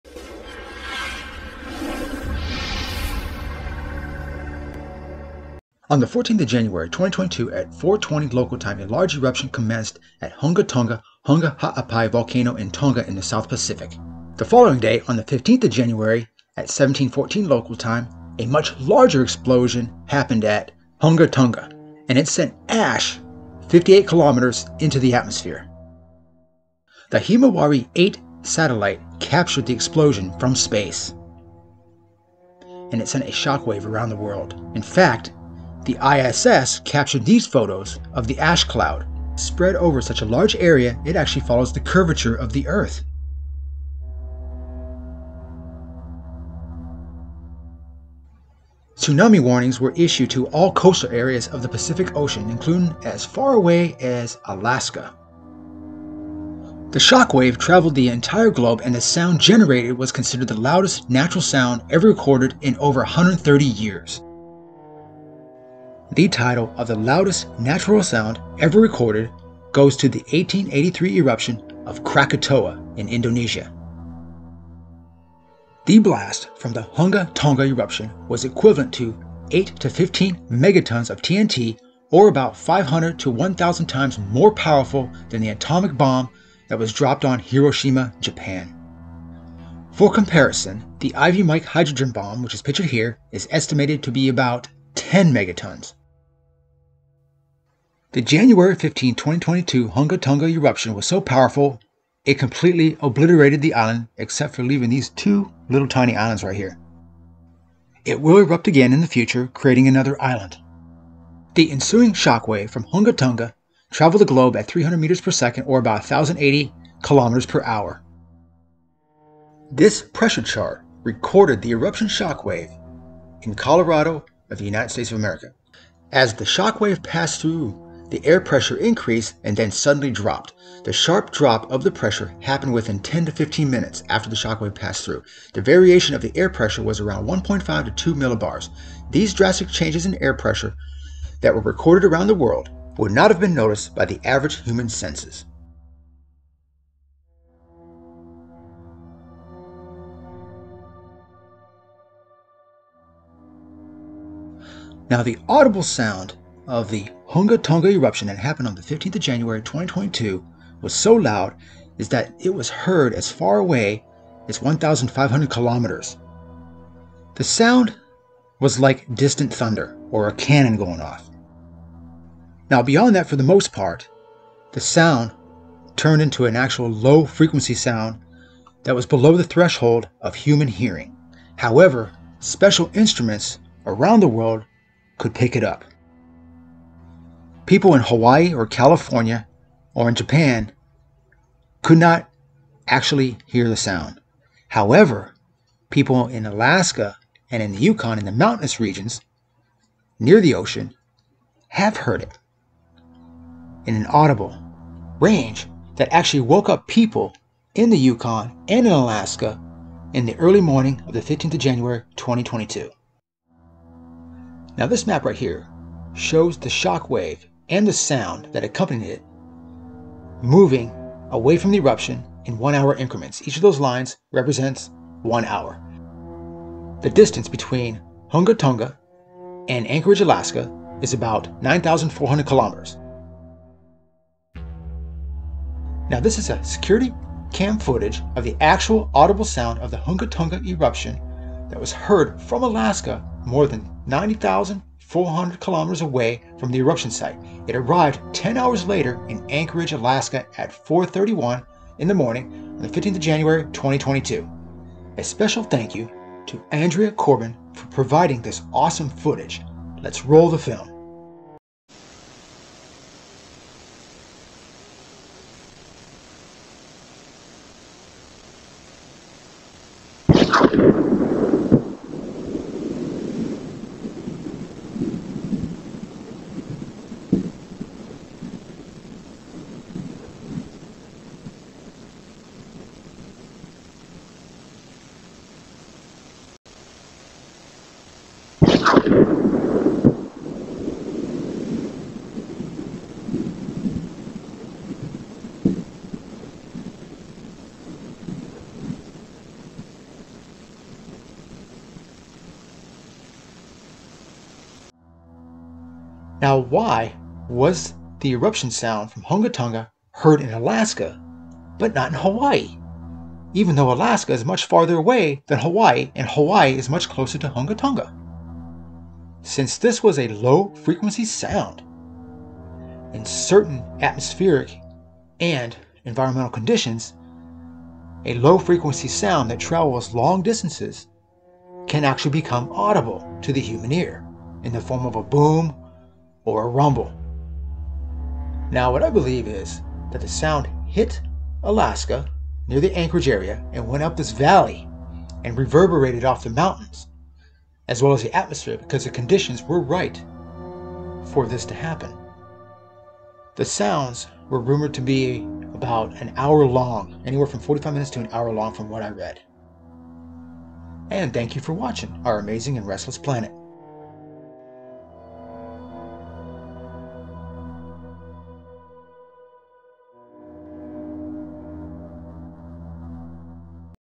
Heard 6000 Miles Away In Alaska! Hunga Tonga Audible Recorded Eruption Sound 1/15/2022
Security camera footage in Anchorage, Alaska recorded the actual audible sound and shockwave of the Hunga Tonga Hunga Ha'apaa eruption 10 hours later in the early morning at 0431 local time on 15 January 2022. A special Thank You to Andrea Corbin for allowing us to use her security cam footage.
The distance of Hunga Tonga from Alaska is around 9400 kilometers or 5800 miles. The sound from the eruption blast travelled around the globe. It could be heard up to 1500 km away. After that it was a low frequency sound (infrasound) too low to be heard by human ears. Only special instruments recorded it. However under the right atmospheric and environmental conditions a low frequency sound can reverberate creating an audible sound. The Hunga Tonga Hunga Ha'apai eruption boom recorded and heard as far aways as Alaska and was also heard in the Yukon.

Hunga Tonga Eruption- Tonga Geological Services
ISS photos- NASA
Security Cam Footage Of Sound- Andrea Corbin
Island Map-NASA
Space GIF of the eruption- Himawari 8 satellite.